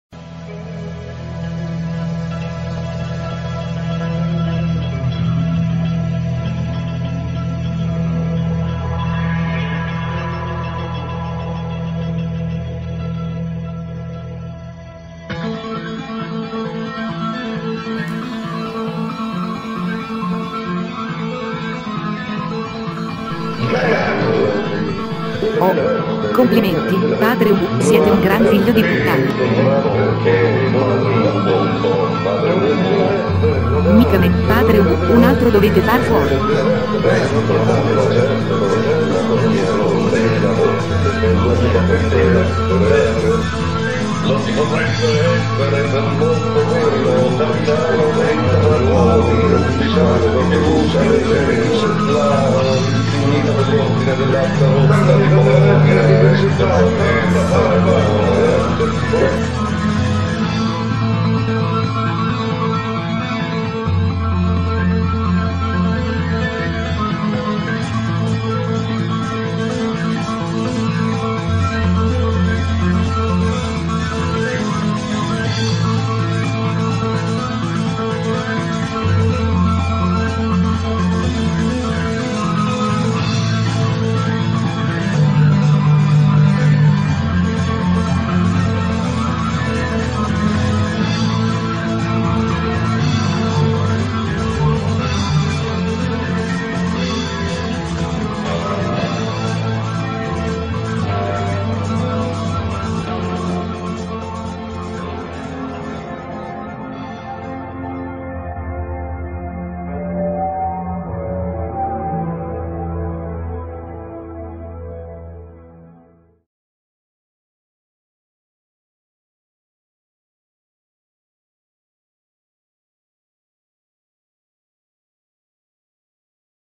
KAZ KontAnymated Zone - Ubu Roi Trailer - kaz.too.it
Trailer of the Imaginary Staging Of Alfred Jarry's "Ubu Roi" by Kontanymated Zone. Video Editing by Valentino De Petro - 3d graphic by Valentino De Petro and Rosario Giacomazza.
On stage from 30 February 2012.